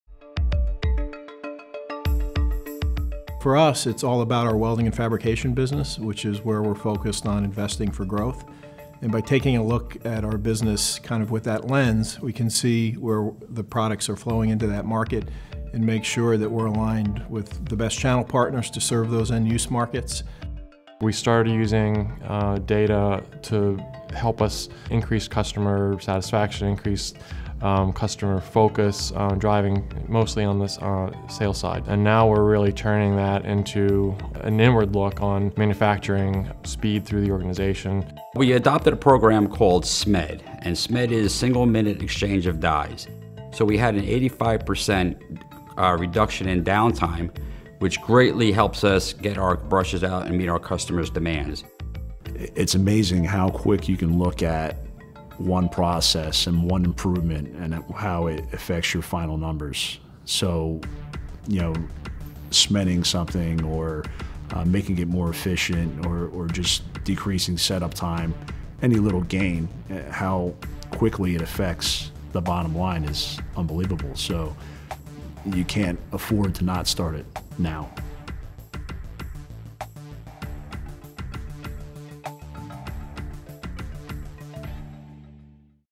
Weiler Corporation fine tunes their manufacturing operations with analytics
Weiler uses analytics to significantly reduce downtime and refine their welding and fabrication processes, improving the bottom line.